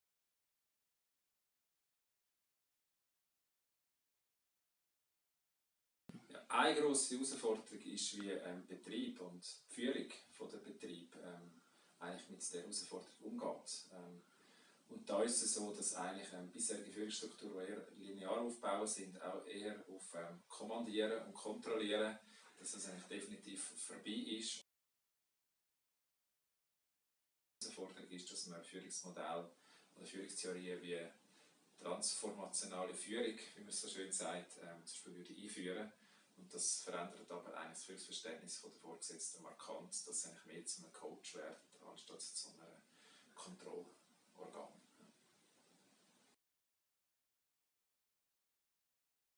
Beat Leuthold Transformationale Führung
Work-Life-Balance 4.0: Was sind derzeitige Problematiken und Herausforderungen einer «Work-Life-Balance 4.0»? In dieser Serie antworten verschiedene Expertinnen und Experten.

In diesem Beitrag kommt Beat Leuthold, Betriebliche Sozialberatung zu Wort.

«Work Life Balance 4.0» ist ein Projekt der FHS St.Gallen. Ziel ist es, mit Akteurinnen und Akteuren aus Wissenschaft, Wirtschaft und Politik idealtypische Gestaltungsprozesse der Arbeitswelt 4.0 und Lösungskonzepte einer Work-Life-Balance 4.0 zu entwickeln. Unterstützt wird das Projekt durch das Eidgenössische Büro für die Gleichstellung (EBG) mit Finanzhilfen nach dem Gleichstellungsgesetz.